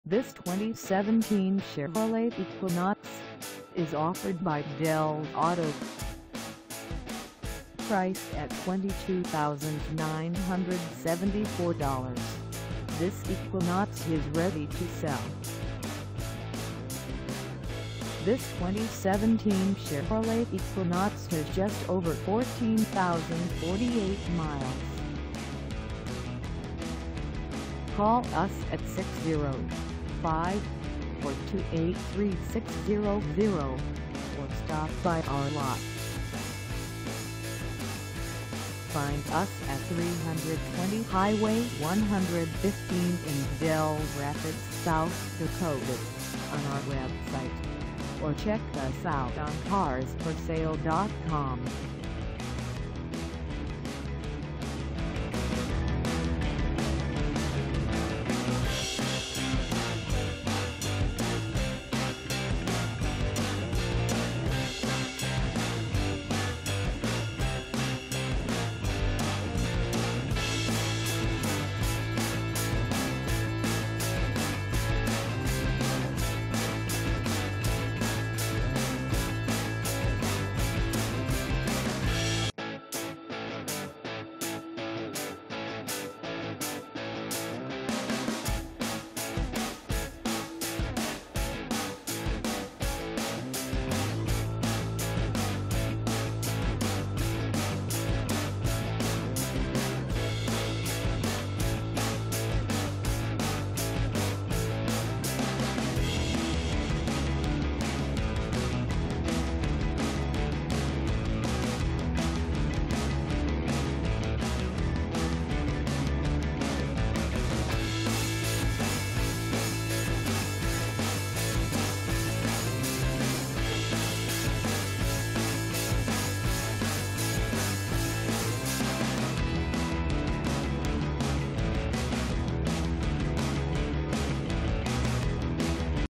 2017 Chevrolet Equinox LT for sale in Dell Rapids, SD 57022
This 2017 Chevrolet Equinox LT is for sale in Dell Rapids, SD 57022 at Dells Auto.

Engine:
Transmission: Automatic 6-Speed
Color: BLACK
Mileage: 14048
Contact Dells Auto 320 Hwy 115 Dell Rapids, SD 57022 to test drive this 2017 Chevrolet Equinox LT today.